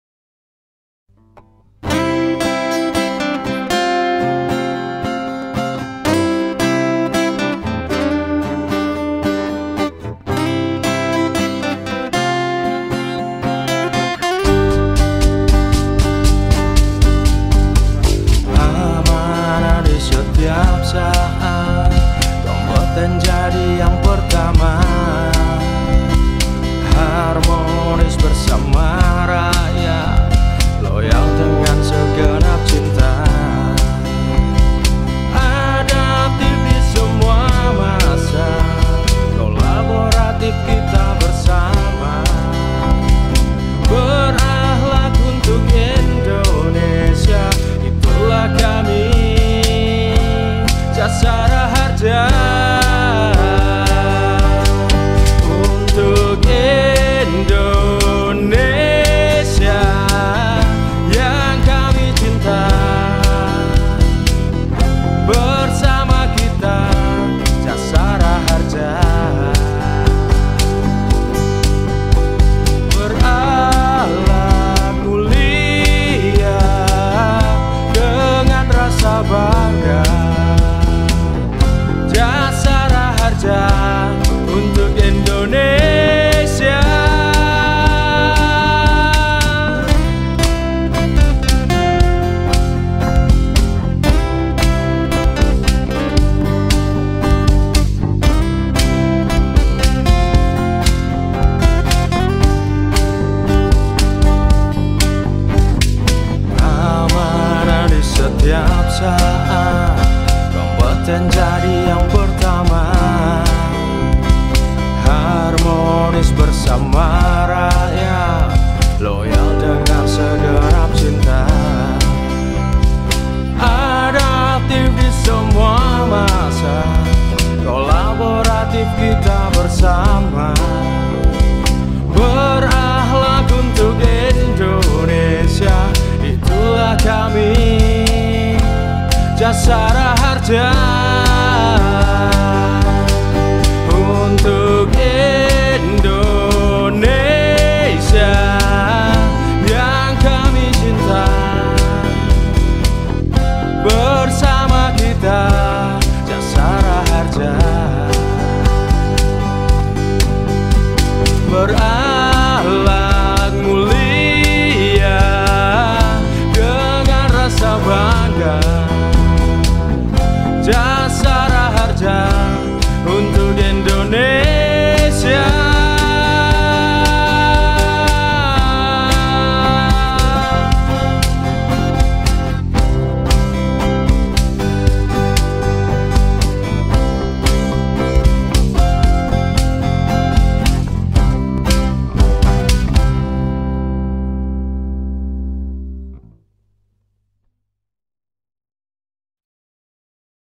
VIDEO PEMENANG JUARA 1 - LOMBA CIPTA LAGU JASA RAHARJA
Malam Penghargaan Lomba Cipta Lagu Jasaraharja dengan Karya Lagu-Lagu Terbaik Dari Insan Jasa Raharja